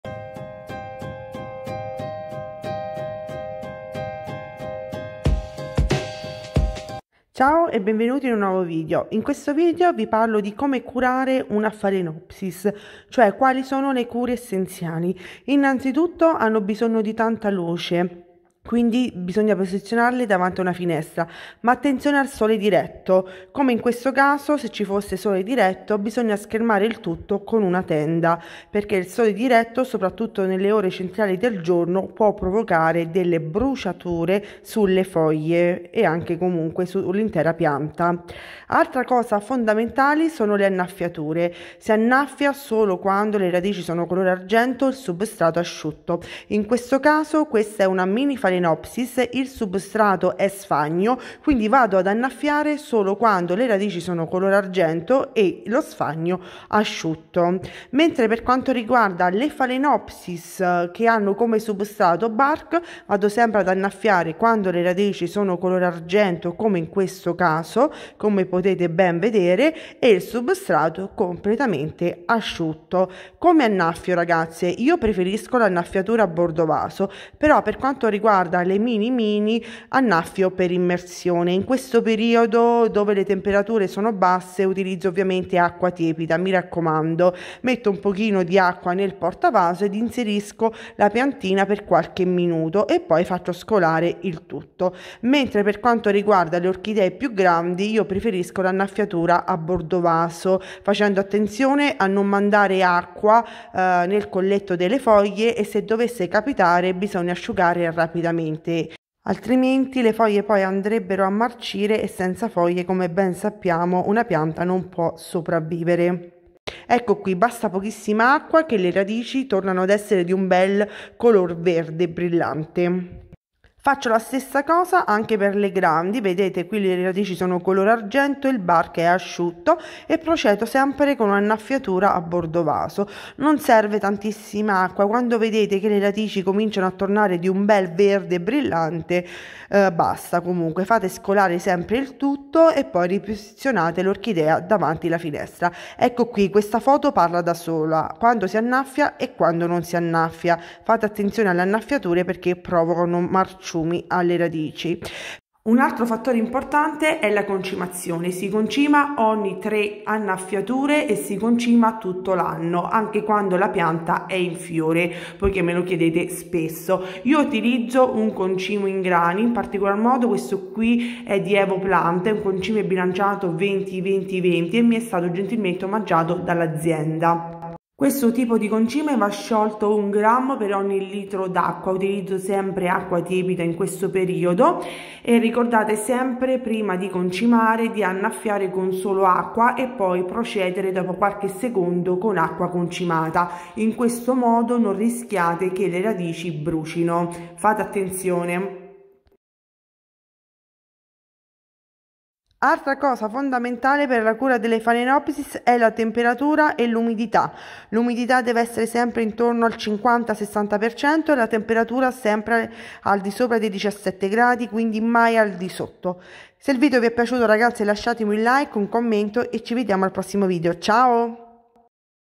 Come curare un'orchidea? 5 consigli per farla rifiorire!
Benvenuti sul mio canale Le orchidee di Gessica, dove troverai consigli sulla cura delle orchidee.

Il concime mostrato nel video mi è stato gentilmente omaggiato da Evoplant.